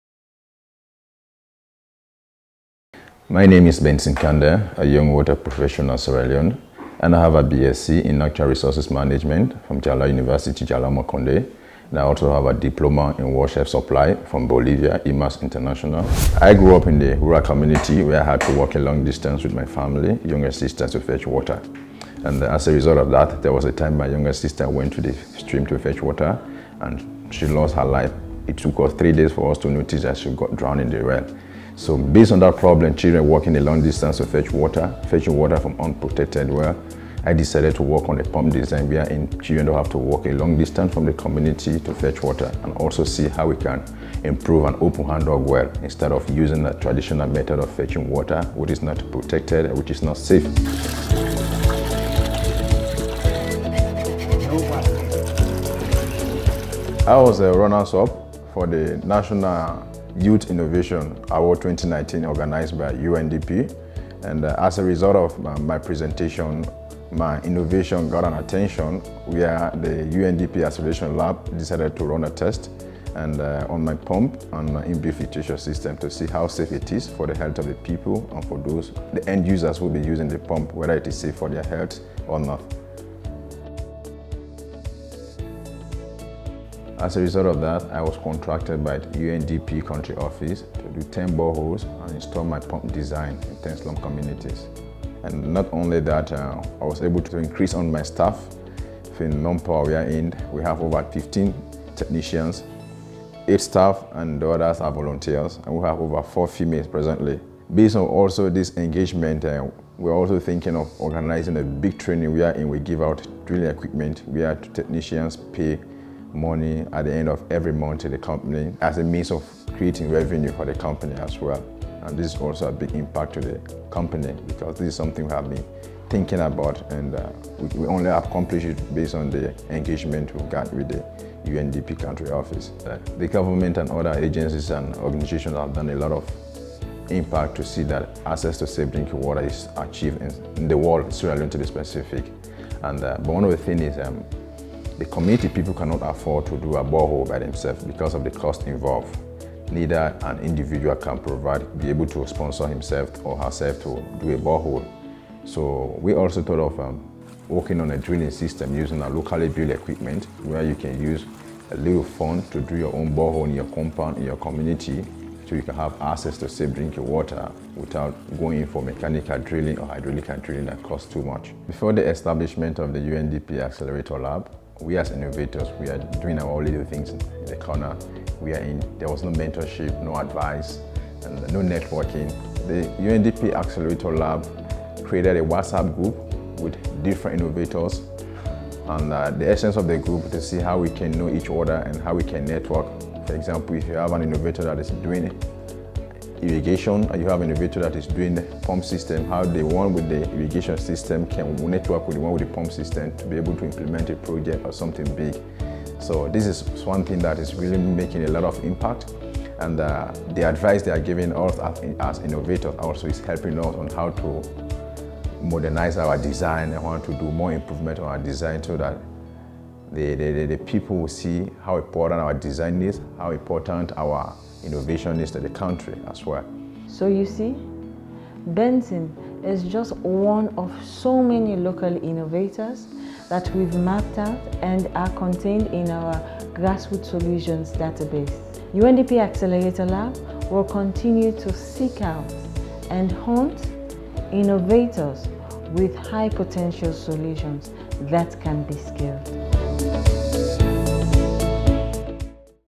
Benson Kandeh Innovator Sierra Leone
Benson Kandeh is a Sierra Leone born Innovator who through his unfortunate personal experiences have designed an improved version of the traditionally known "open hand dug wells" using self designed tools and techniques to drill low tech and low cost boreholes that can be easily maintained.
This solution brings much needed value to local communities as access to safe drinking water remains their major challenge. His design is not only 1) cost effective for both initial installation and maintenance as compared to other options 2) It comes with an in-built locally designed water filtration system 3) Is completely sealed off to discourage contamination  from external elements  4) Very user friendly as both adults and kids can use easily 4) With little/no possibility for fatal accidents to occur which cannot be said of the traditionally designed wells.
Seeing the potential of the solution the UNDP Accelerator Lab in Sierra Leone has been working with Benson to explore means through which his solution could be scaled after series of water testing has been done successfully.
After series of tests were conducted and the results were declared safe  for domestic use, UNDP in Sierra Leone awarded him his first contract to roll out his design in 10 slum communities who were in dire need of safe water for domestic use during the early stages of the Corona pandemic in Sierra Leone.